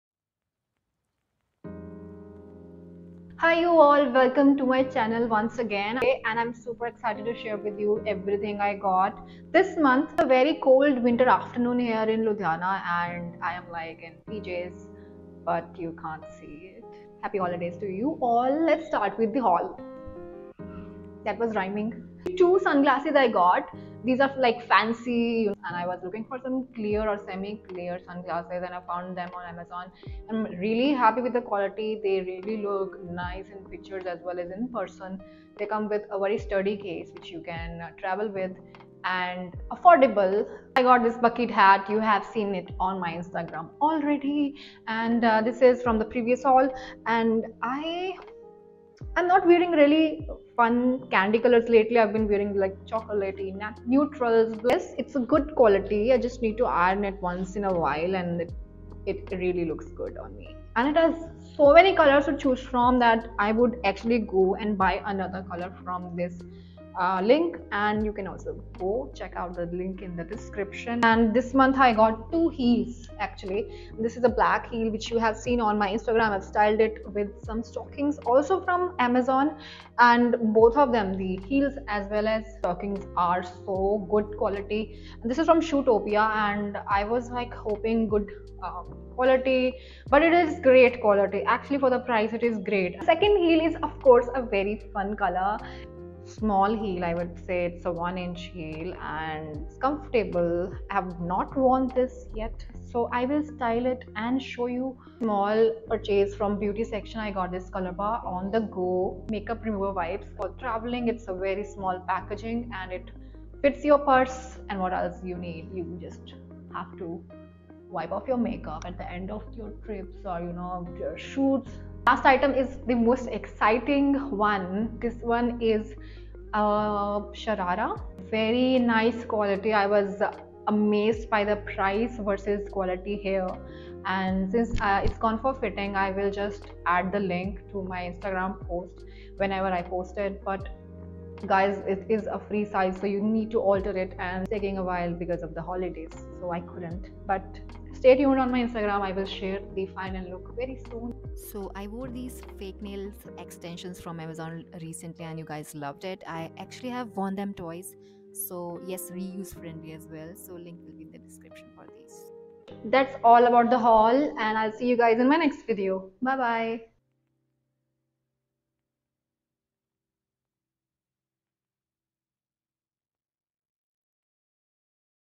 Amazon DEC Haul Part 2 | Ocean Thakur
Hi you all Hope you liked the video. Here are the links to the haul items-
Black heels
Green heel
Grey Sunglasses
Bucket Hat
Beige Sunglasses
French nails

or DM on instagram- oceanthakur

CHECK MY BUSINESS - BEAUTYNATION - International makeup house that delivers Makeup and Skincare all over India

Editor I use
Tripod I Use